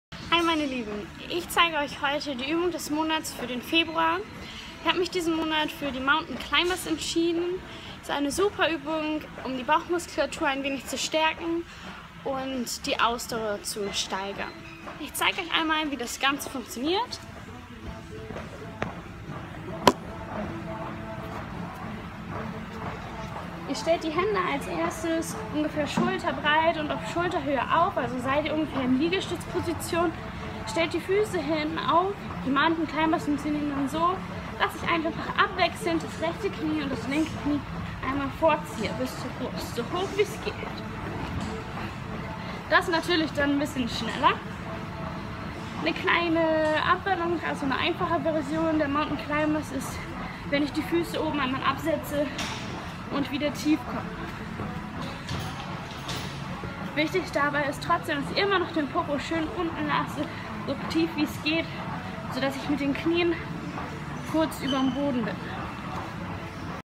Fitness in Wildeshausen Gym80
Übung des Monats Februar
Mountain Climbers mit Svenja